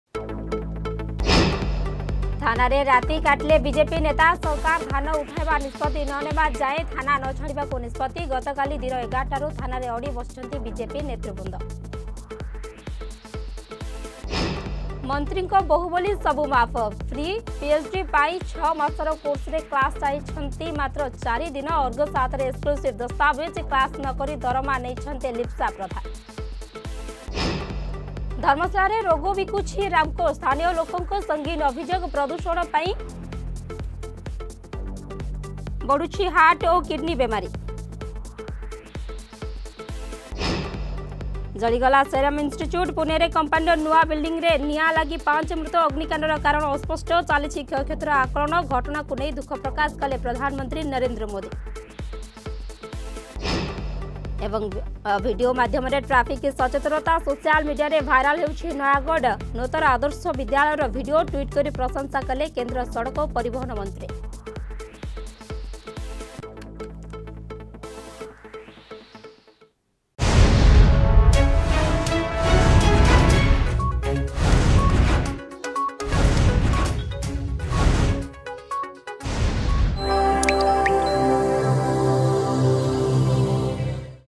Headlines 8am
ଥାନାରେ ରାତି କାଟିଲେ ବିଜେପି ନେତା...
ମନ୍ତ୍ରୀଙ୍କ ବୋହୂ ବୋଲି ସବୁ ମାଫ୍.
ଧର୍ମଶାଳାରେ ରୋଗ ବିକୁଛି ରାମକୋ .
ଜଳିଗଲା ସେରମ ଇନଷ୍ଟିଚ୍ୟୁଟ...
ଭିଡ଼ିଓ ମାଧ୍ୟମରେ ଟ୍ରାଫିକ ସଚେତନତା...